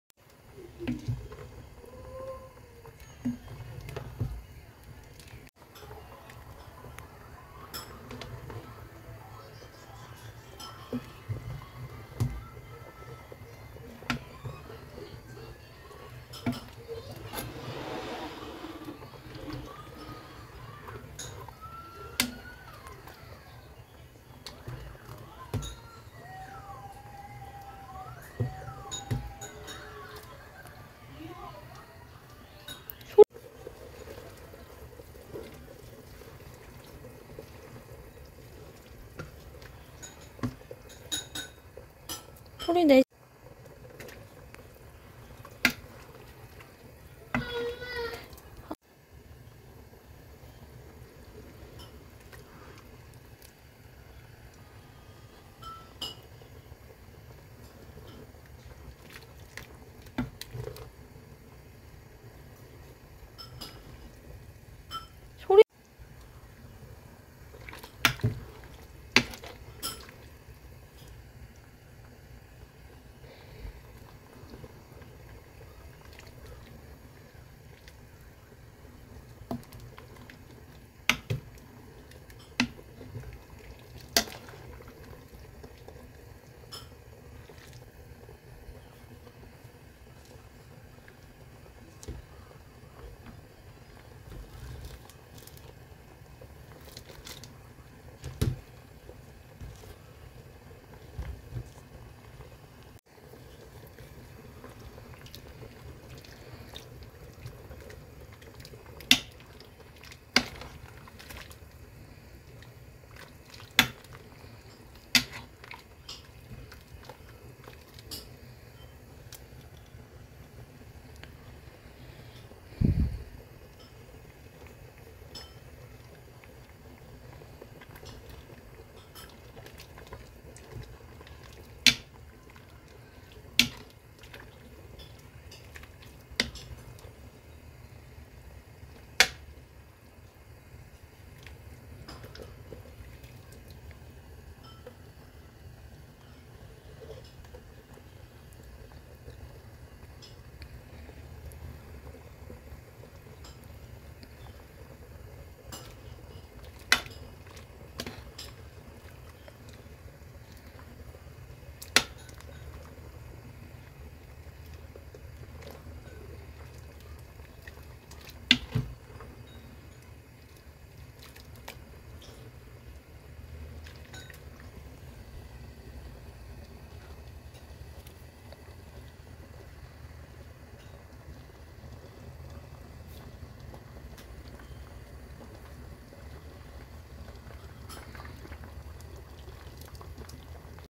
💕곱창전골💕
💕곱창전골 맛집에서 포장해 왔어요.저녁식사 들 맛있게 드시고요.행복한 저녁시간 보내세요. 💕